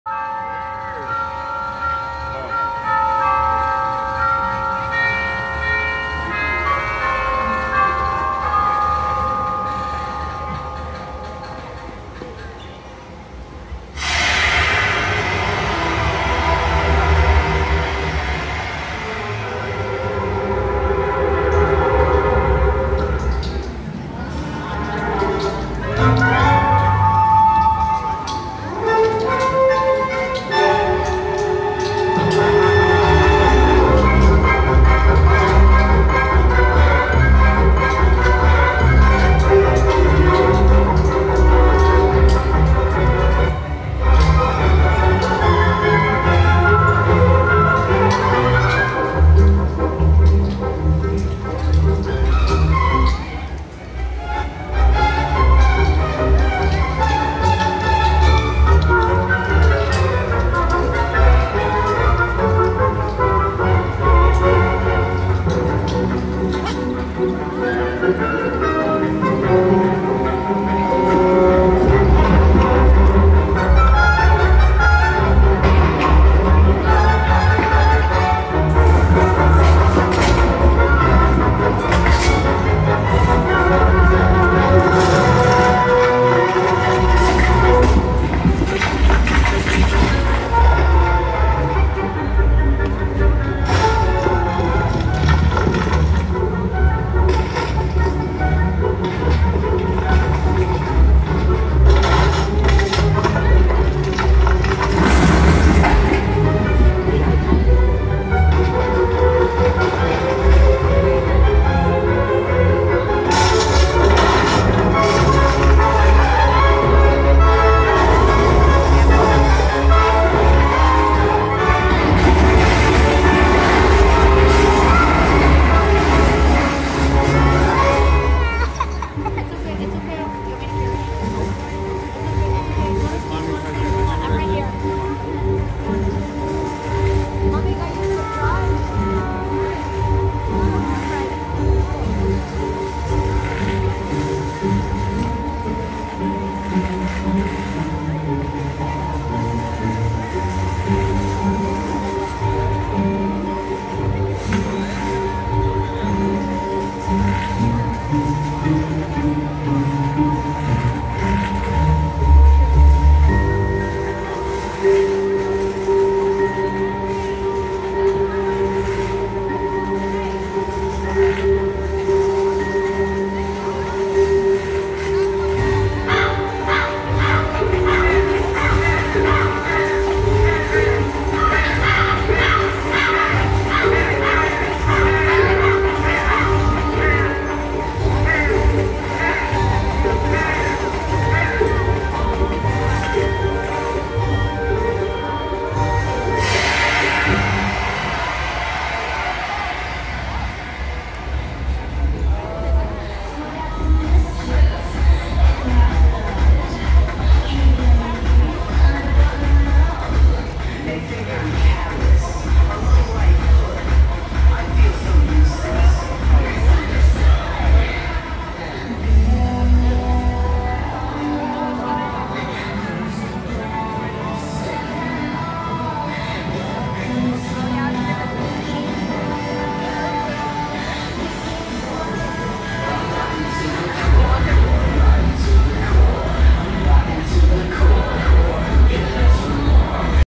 All Hallows' Eve Celebration 4K Show - Oogie Boogie Bash - Disney California Adventure - Sept. 2024
All Hallows' Eve Celebration - Oogie Boogie Bash - Disney California Adventure

This video was recorded at Carthay Circle inside Disney California Adventure Park on September 29, 2024 during the 11:00 PM show.